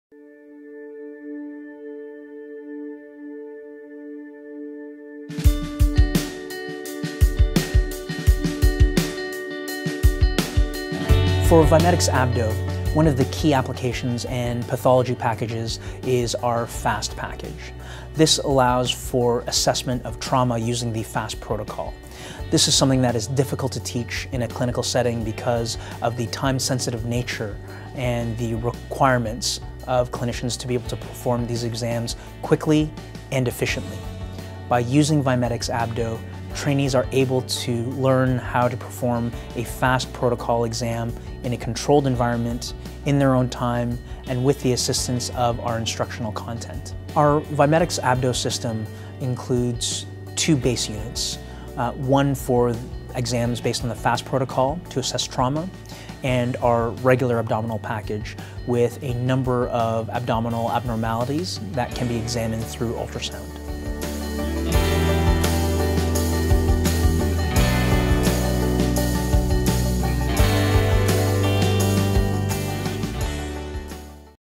腹部超音波教學模擬 Vimedix 系列 | 多種病例內容，由鎖骨至膀胱全範圍連續超音波掃描
隨時隨地學習腹部超音波相關技能，並且全程無需與真實病人接觸，無任何風險。操作超音波探頭進行臟器掃描，以及解讀掃描得來的超音波影像。顯示器能同時展示臟器解剖圖和即時超音波掃描圖像，從而向學員提供身臨其境的虛擬超音波學習體驗，使學員用最短的時間熟悉超音波技能，達到訓練目的。